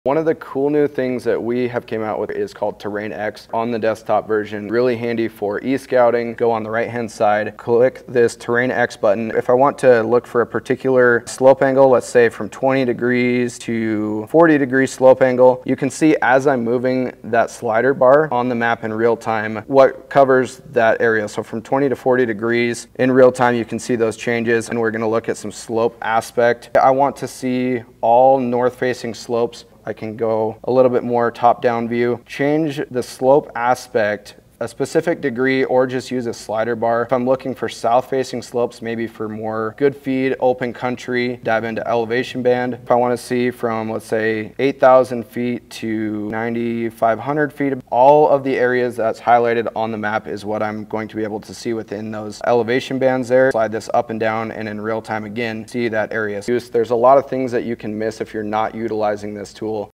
E-Scouting Just Got A Lot Better!
Try the new Terrain-X feature within the onX Hunt App when you're preparing for your upcoming hunts this season! Available for onX Hunt Elite!